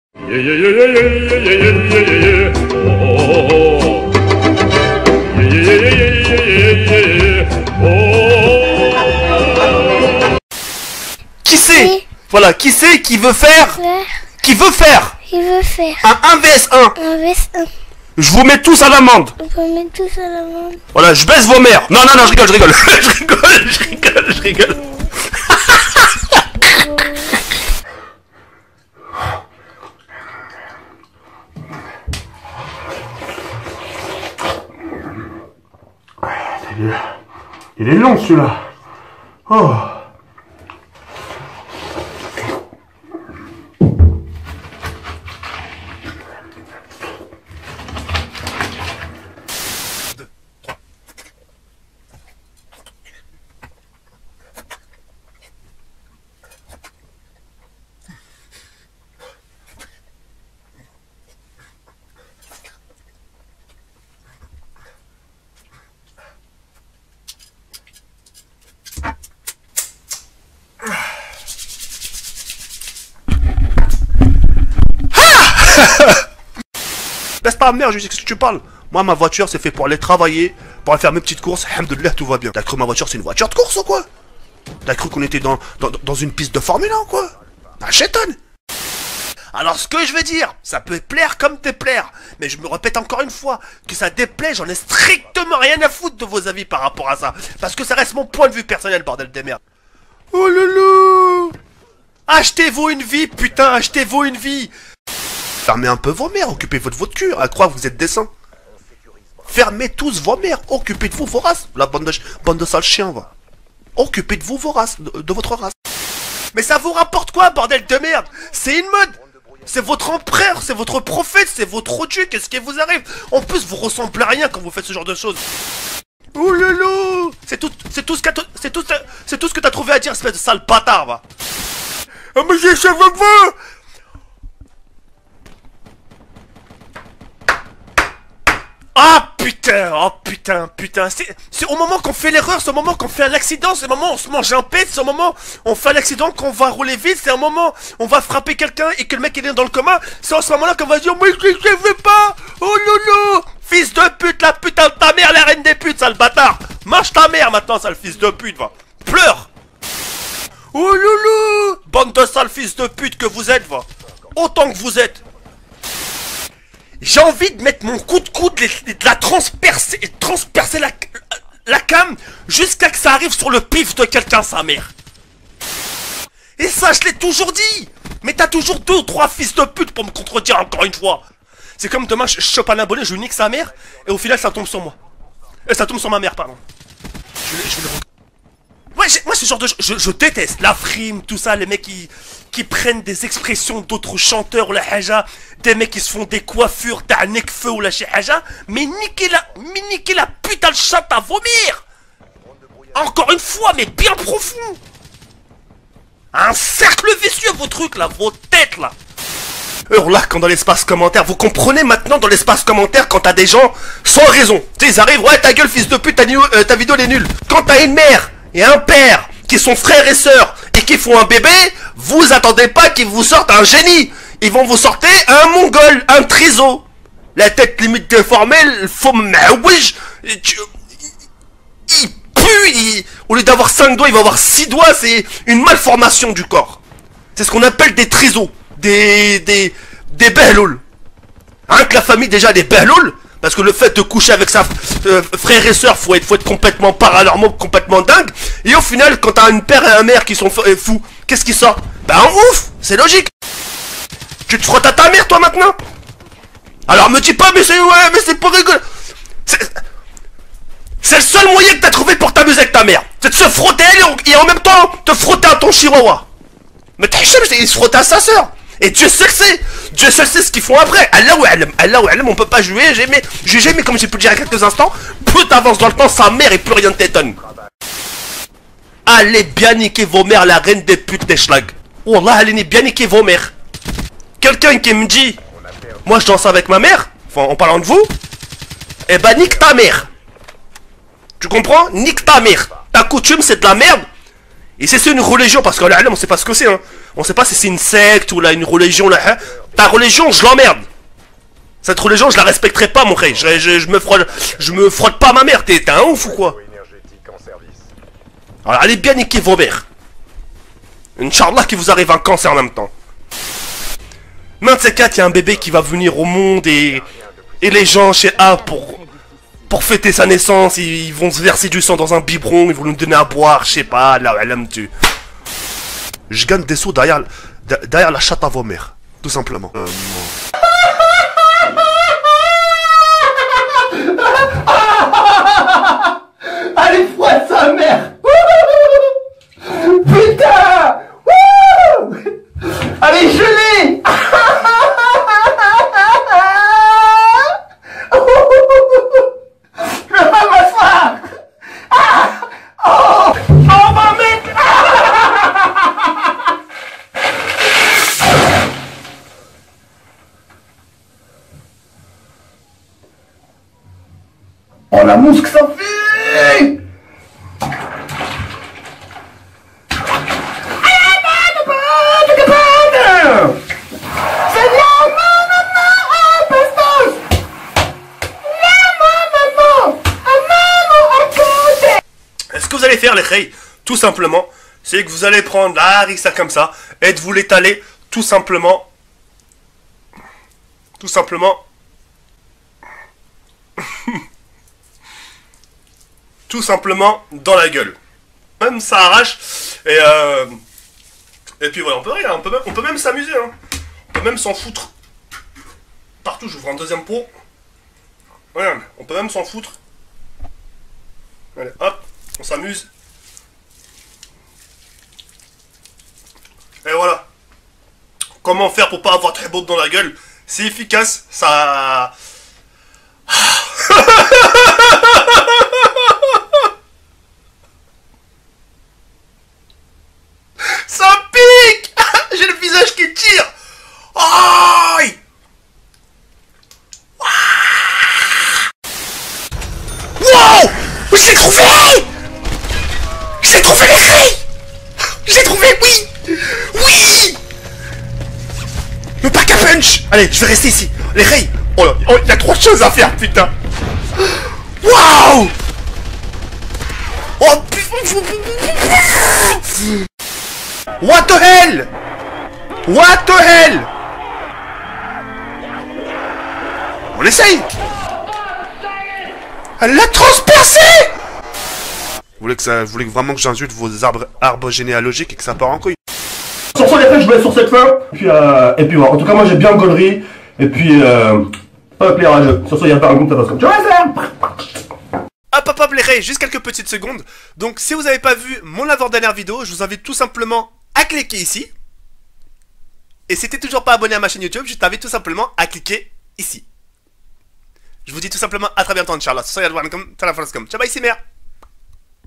Best Of TheKairi78 : Mes pire insultes !!
Salem les rhey voilà un petit Best of spéciale insulte qui m'a été fais par un abonné je vous souhaite un bon visionnage.